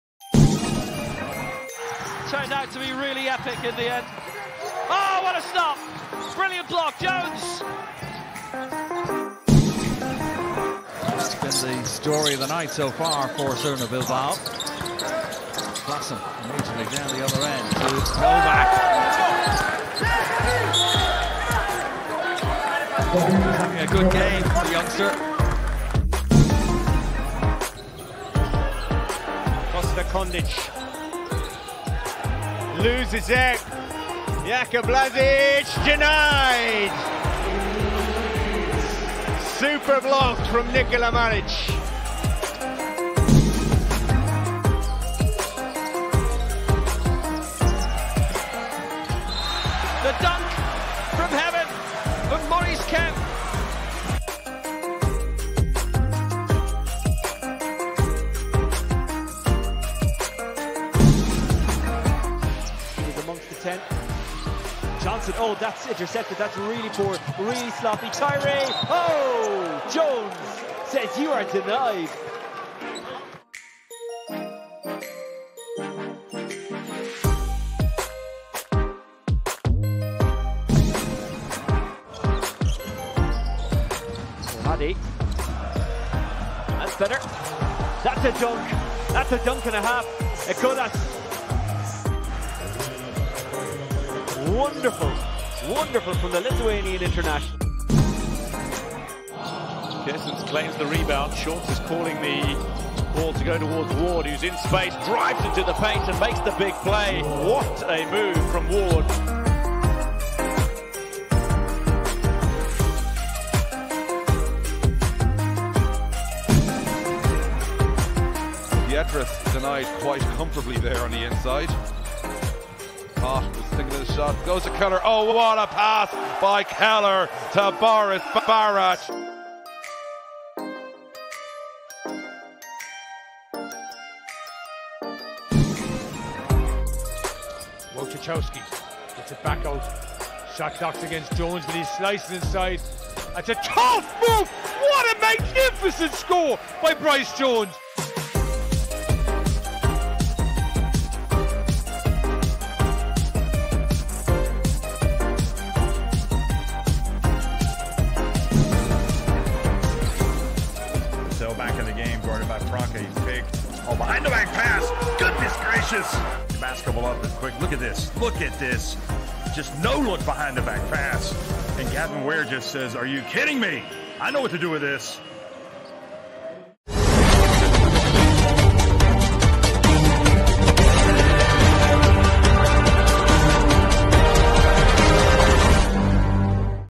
Top 10 Plays | Week 10 - Basketball Champions League
Check out the top 10 plays from Week 10 of the Basketball Champions League 2022-23

1 - Assist by Jonathan Rousselle - JDA Bourgogne Dijon
2 - 2-pointer by Bryce Jones - Limoges CSP
3 - Assist by Akos Keller - Falco Vulcano Szombathely
4 - Dunk by Tyson Ward - Telekom Baskets Bonn
5 - Dunk by Martynas Echodas - Rytas Vilnius
6 - Block by Marvin Jones - Hapoel Atsmon Holon
7 - Dunk by Maurice Kemp - Bnei Ofek Dist Herzliya
8 - Block by Nikola Maric - Igokea m:tel
9 - Dunk By David Novak - ERA Nymburk
10 - Block by Jamal Jones - Dinamo Bds Sassari